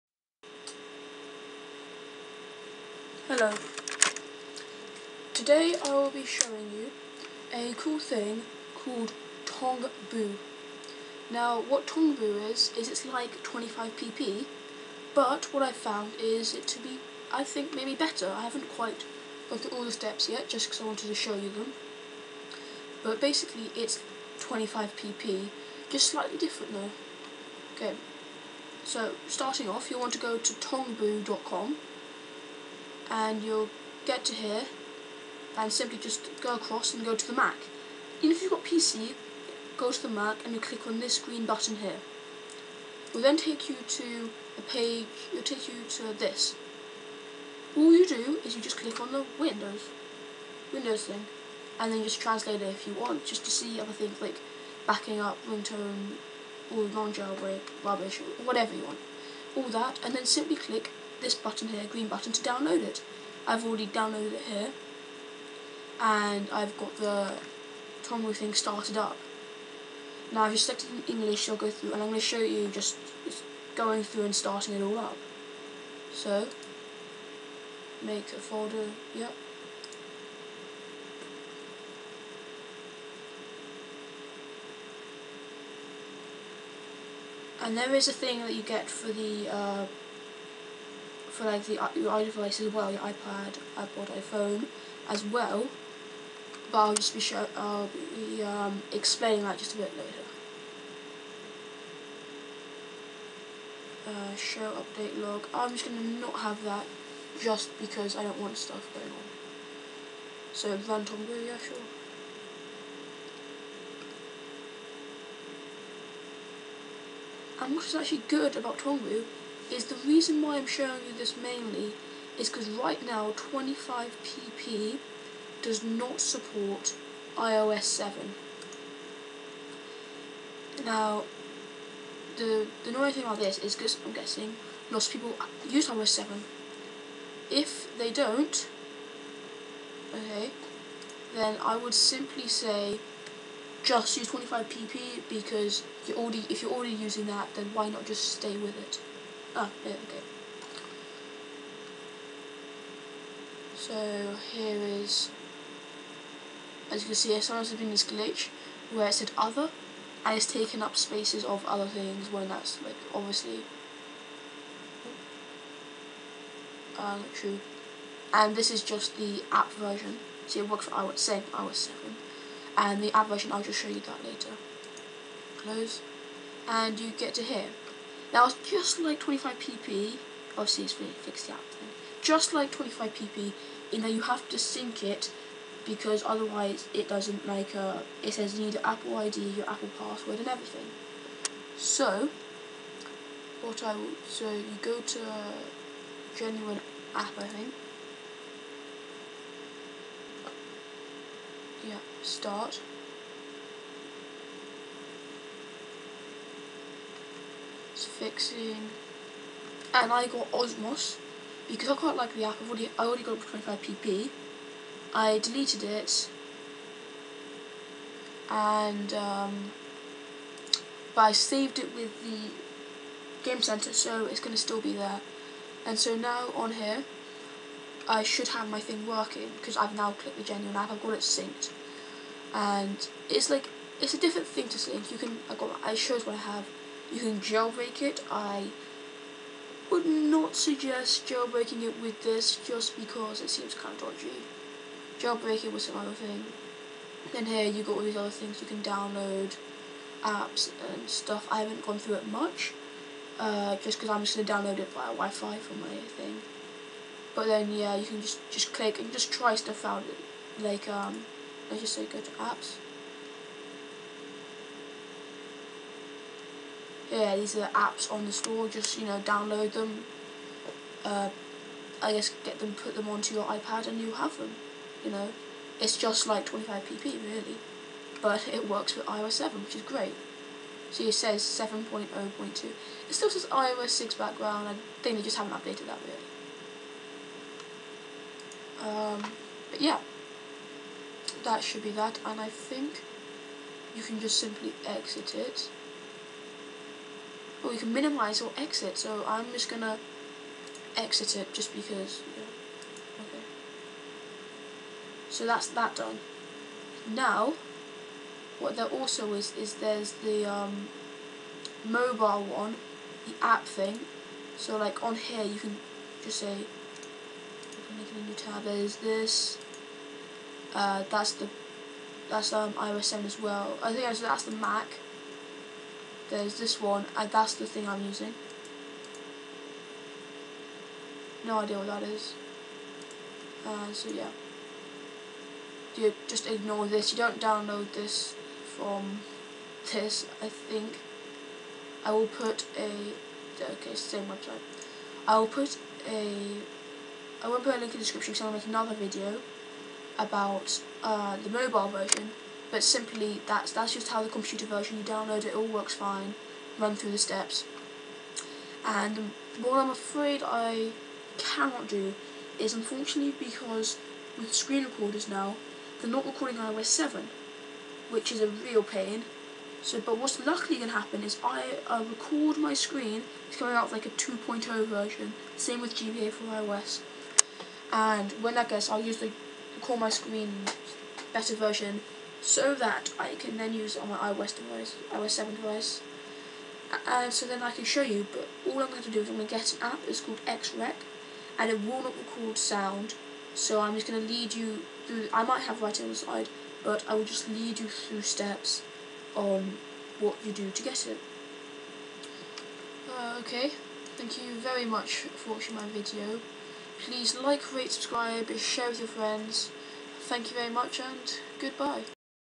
Tongbu - Getting Free Apps on an iDevice (no jailbreak) for iOS7: Part 1
In this video I will show you the computer version of Tongbu and how to set it up.